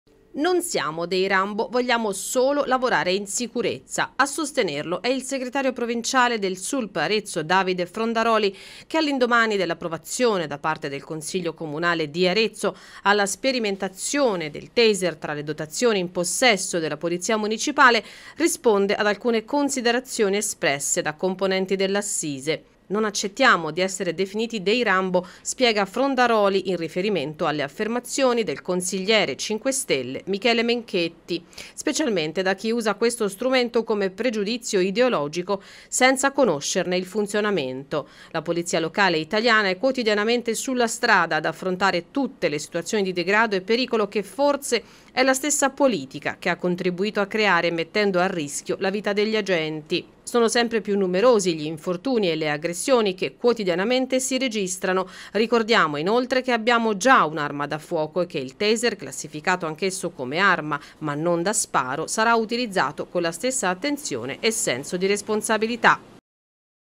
"Non siamo dei "Rambo". Il Sulpl provinciale risponde a Menchetti (Movimento 5 Stelle)
"Non siamo dei "Rambo" vogliamo solamente lavorare in sicurezza".
A sostenerlo è il segretario provinciale Sulpl Arezzo, Davide Frondaroli che, all'indomani dell'approvazione da parte del consiglio comunale alla sperimentazione del taser tra le dotazioni in possesso della polizia municipale, risponde ad alcune considerazioni espresse dai componenti dell'assise. "Non accettiamo di essere definiti dei “Rambo” - spiega Frondaroli in riferimento alle affermazioni del consigliere Menchetti - specialmente da chi usa questo strumento come pregiudizio ideologico senza saperne il funzionamento. La polizia locale italiana è quotidianamente sulla strada ad affrontare tutte quelle situazioni di degrado e pericolo che forse è la stessa politica che ha contribuito a creare, mettendo a rischio la vita degli agenti. Sono sempre più numerosi gli infortuni e le aggressioni che quotidianamente si registrano. Ricordiamo inoltre che abbiamo già un’arma da fuoco e che il taser, classificato anch’esso come arma comune non da sparo, sarà utilizzato con la stessa attenzione e senso di responsabilità. Detto ciò riteniamo che dal punto di vista della sicurezza, quello raggiunto in consiglio comunale, sia un ottimo traguardo raggiunto per la categoria, e pertanto ci tengo a ringraziare il Sindaco, il Comandante e tutta l'amministrazione".